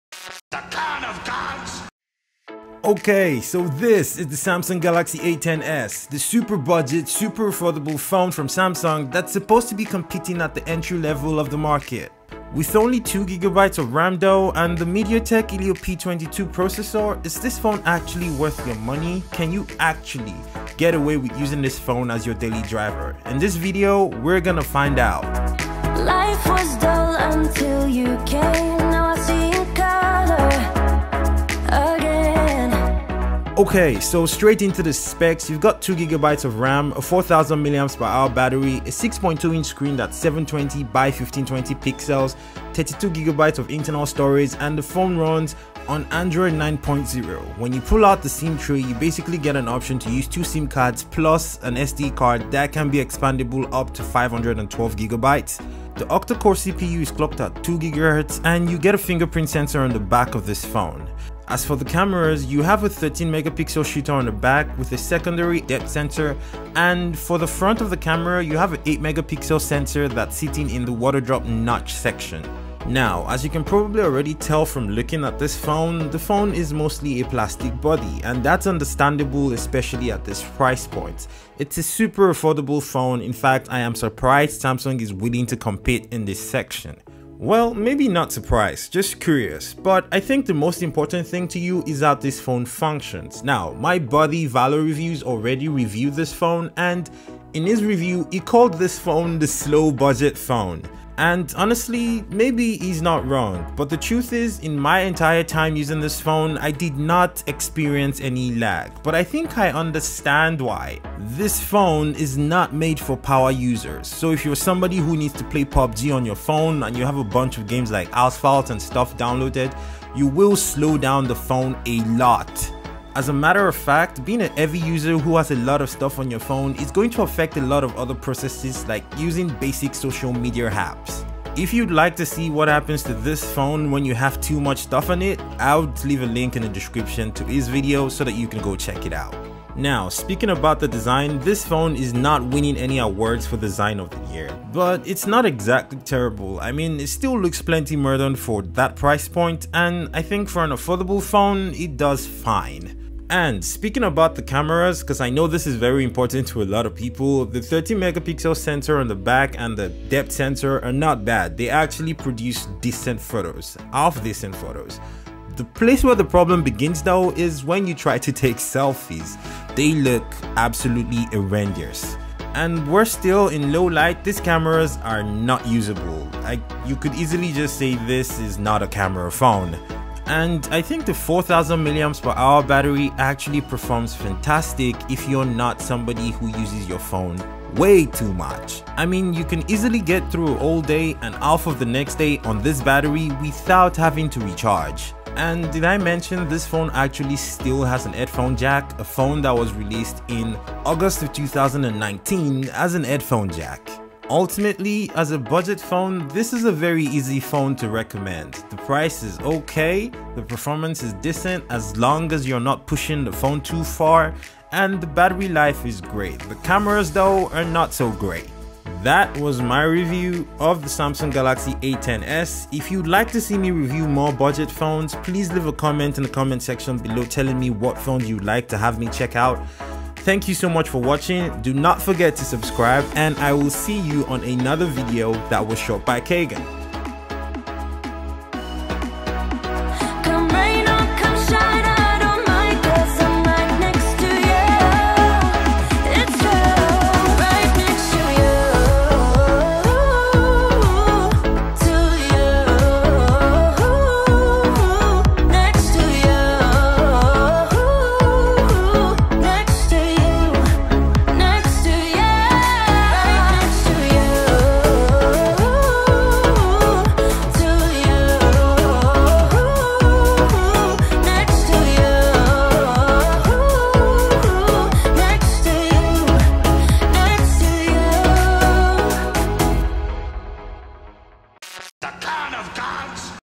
Samsung Galaxy A10s Review - Is This Thing Even Usable
Samsung Galaxy A10s

Samsung Galaxy A10s is a super budget device from samsung. Most likely intended to compete in 3rd world countries like Nigeria, Pakistan, India and so on. The main competition at these extreme budget prices are Transsion Mobile's Tecno & Infinix. So is samsung's low budget offering worth buying?

My Main Camera
My Favorite Fuji Lens
My Secondary Lens
My Third Lens

All the gear I use for my videos

Kagan